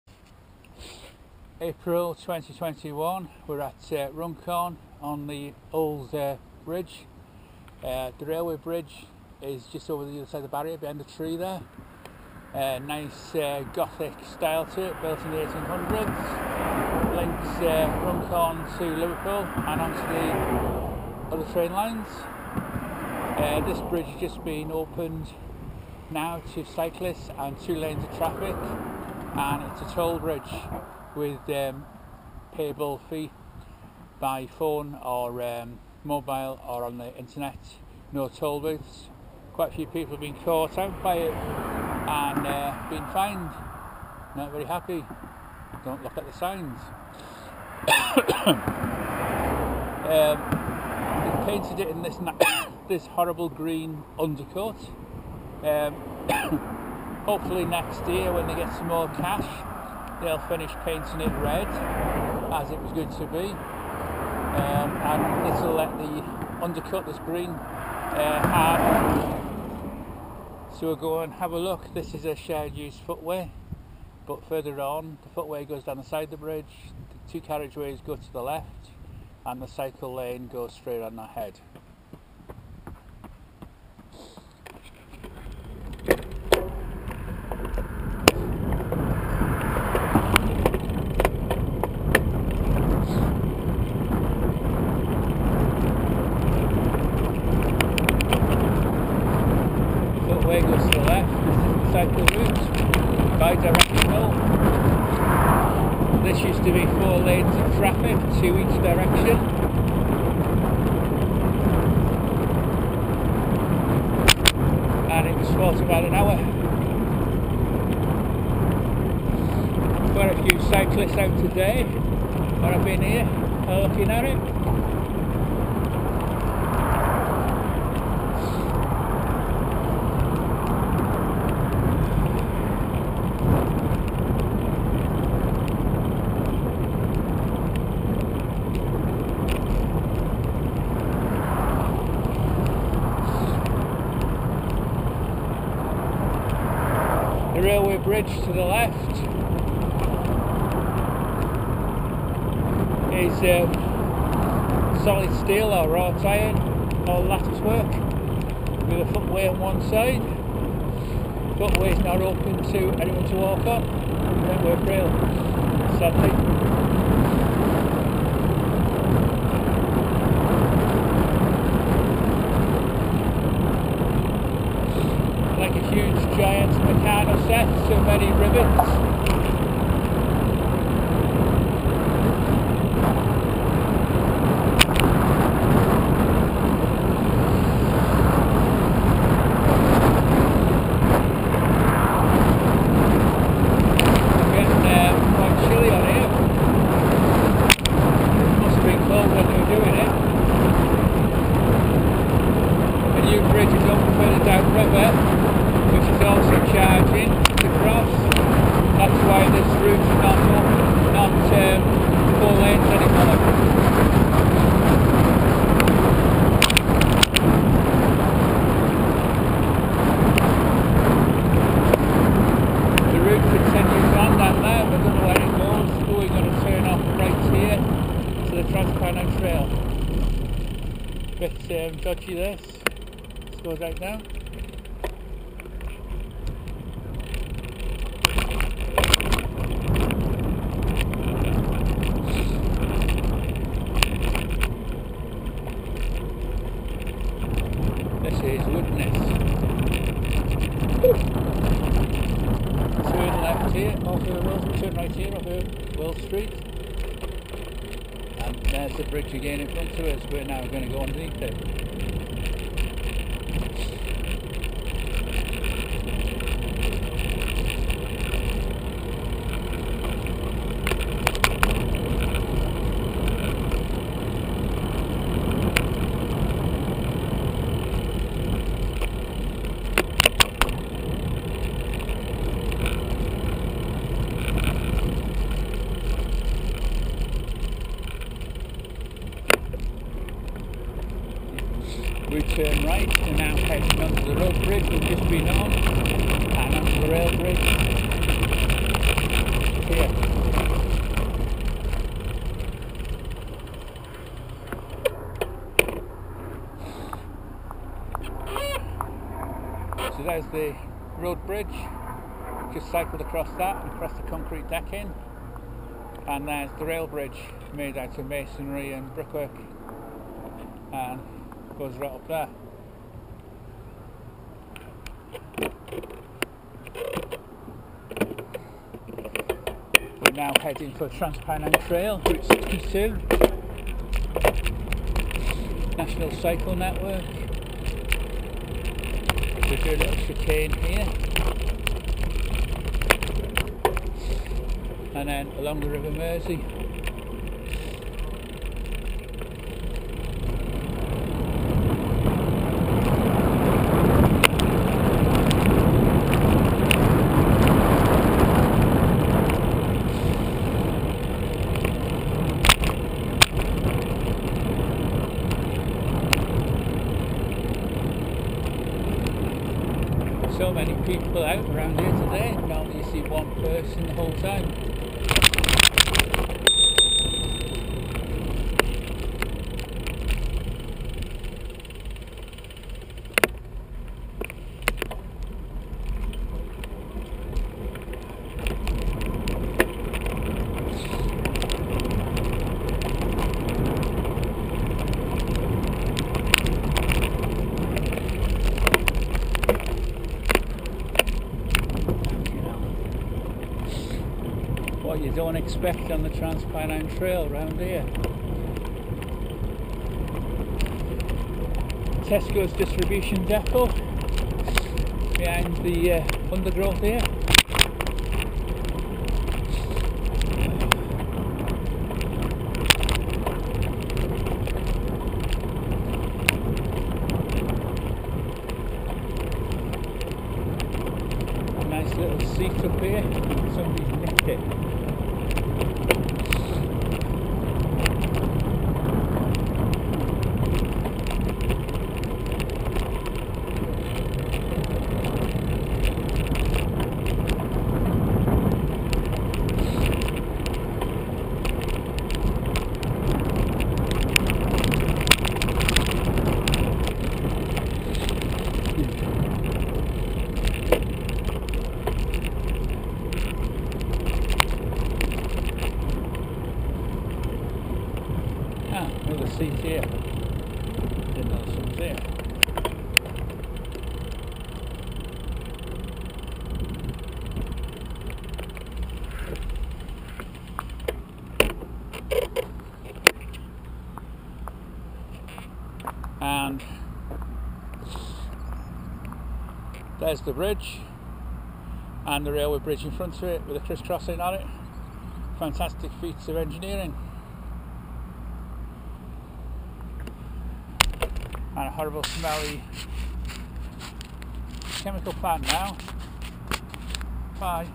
RUNCORN WIDNES BRIDGE OVER AND UNDER, CYCLE ROUTE APRIL 2021
So we start off at Runcorn  and head to Widnes over this fantastic steel riveted and bolted bridge, with a new segregated cycle lane, the bridge was previously 4 lanes of traffic, but a new bridge has been opened to the right, so now we have 2 lanes of traffic and a cycle lane with Pedestrians using the footway to the other side of the main structure.
We then cycle down to under the bridge  and onto the Trans Penine Trail Southport to Hull, via Liverpool and lots of other places in between, a lovely route.